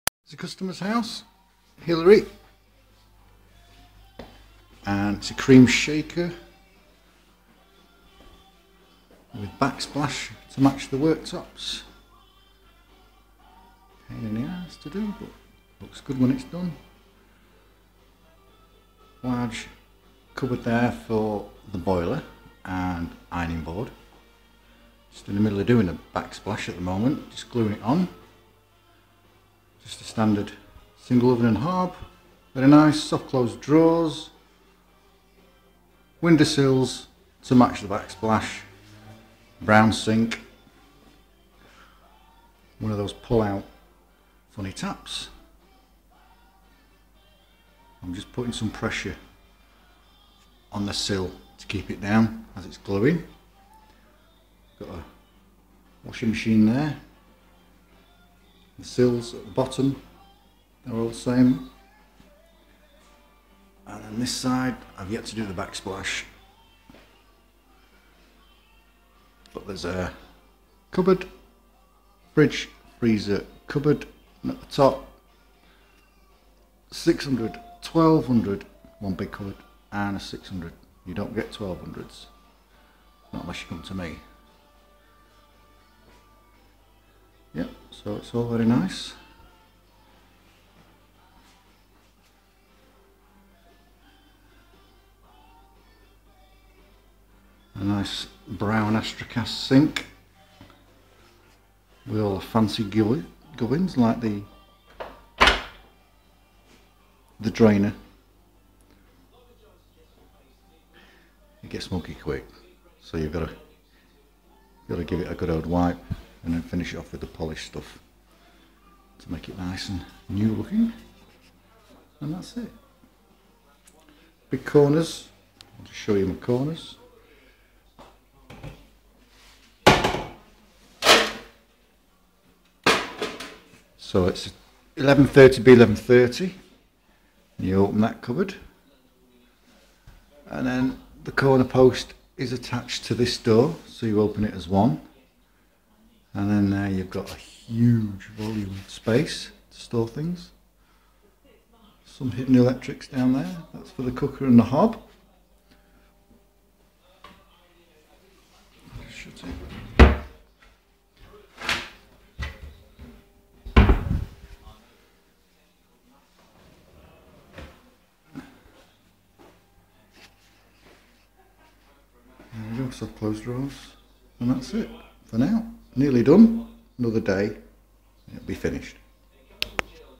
Cream Shaker Kitchen by LCC
One of my kitchens thats on-going, couple of hours work left to do. Backsplash to match worktops.